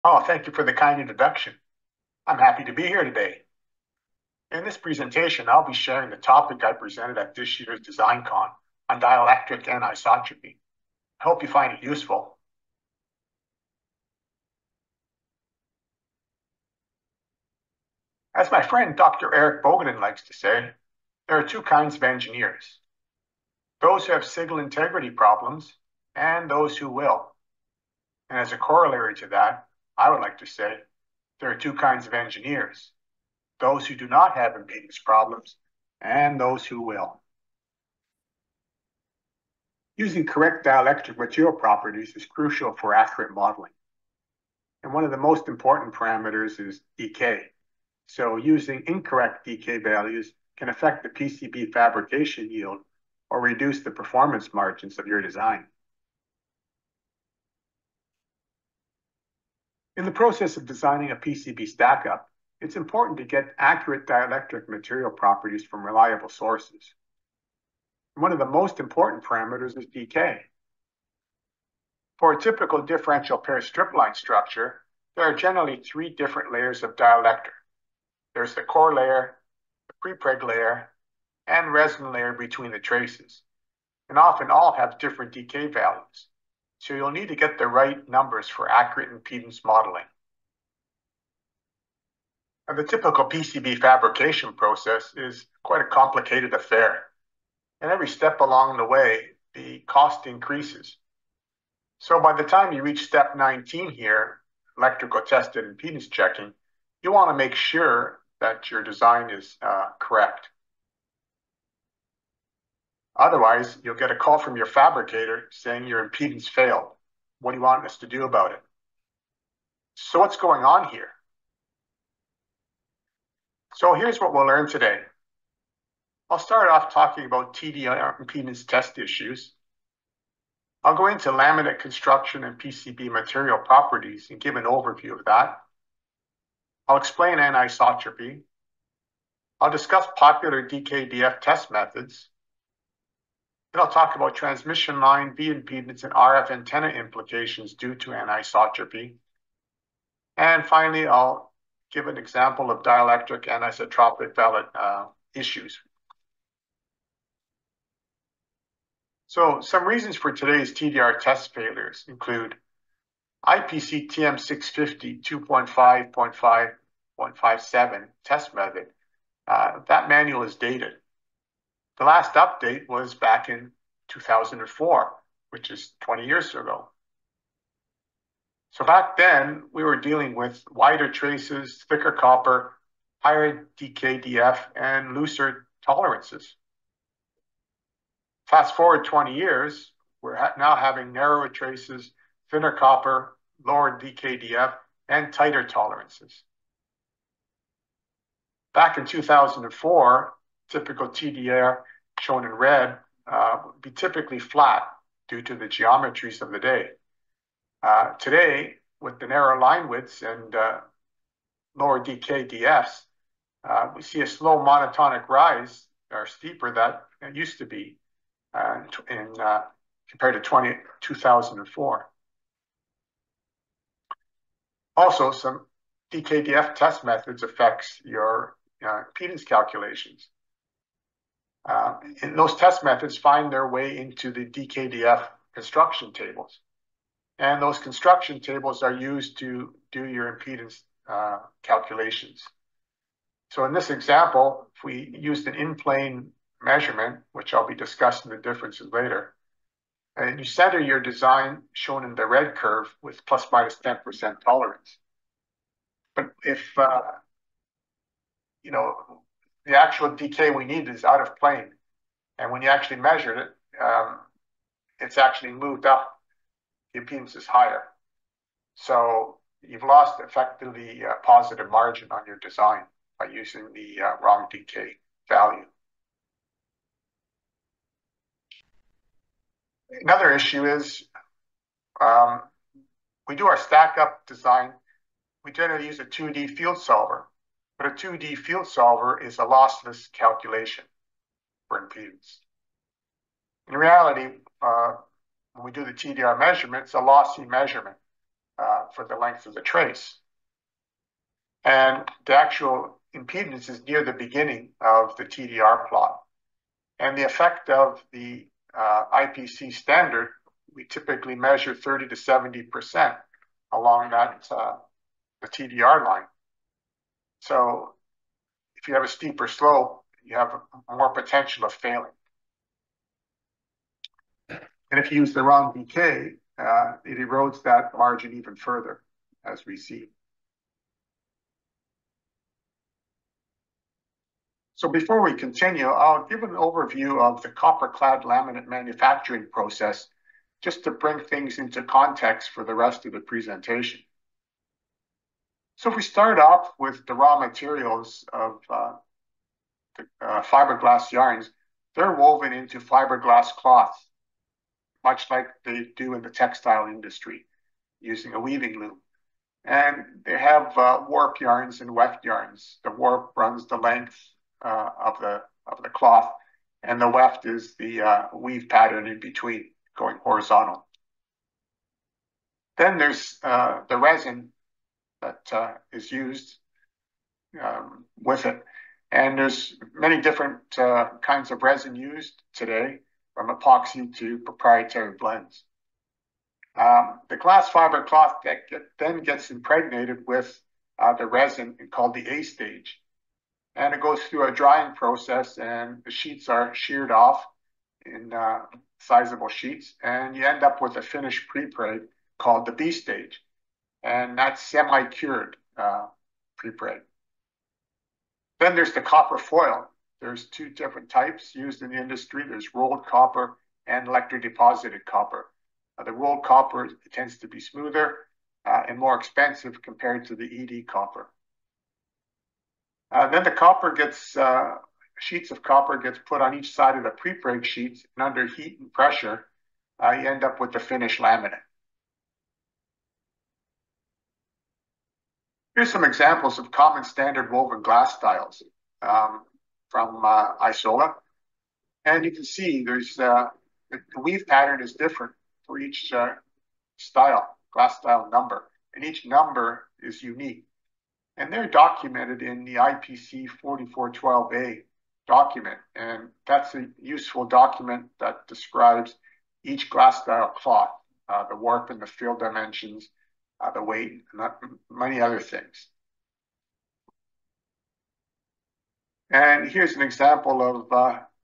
Dielectric Anisotropy Implications for Transmission Line Impedance & Via Modeling | Sierra Circuits
Accurate modeling of transmission lines and vias in PCB design hinges on obtaining reliable dielectric material properties. Among these properties, the dielectric constant (Dk) stands out as a critical factor. Utilizing incorrect Dk values can introduce errors in impedance calculations, posing potential risks to PCB yield and performance. Complicating matters further is the anisotropic nature of fiberglass reinforced laminates commonly used in PCB construction. This anisotropy results in variations in the effective Dk depending on the direction of signal propagation relative to the X, Y, or Z axis. This webinar offers an in-depth exploration of dielectric anisotropy, its implications for transmission line and via modeling, and the possible performance ramifications of incorporating erroneous values into the model.

This webinar is presented by Bert Simonovich.